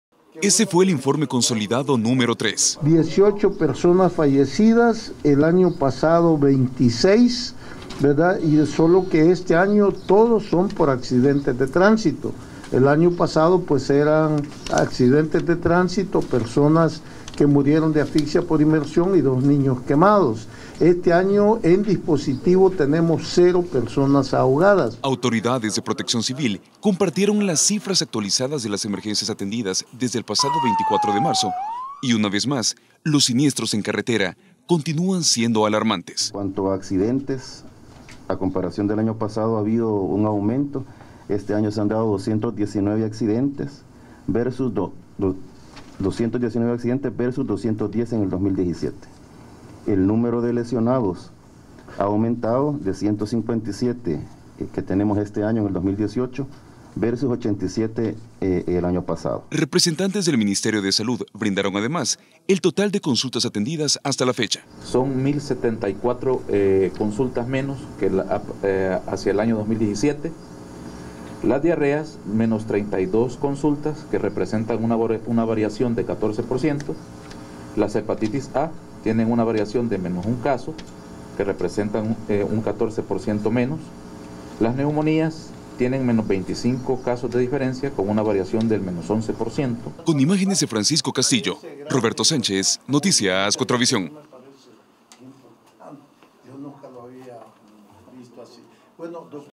Balance Protección Civil, miércoles 28 de marzo.
Los accidentes de tránsito mantienen en alerta a las autoridades y no es para menos, las 18 víctimas en estas vacaciones, han muerto en percances viales, por lo que se hizo un nuevo llamado a la población a tener más precaución. Aquí los detalles del balance de las emergencias atendidas hasta ayer, miércoles 28 de marzo.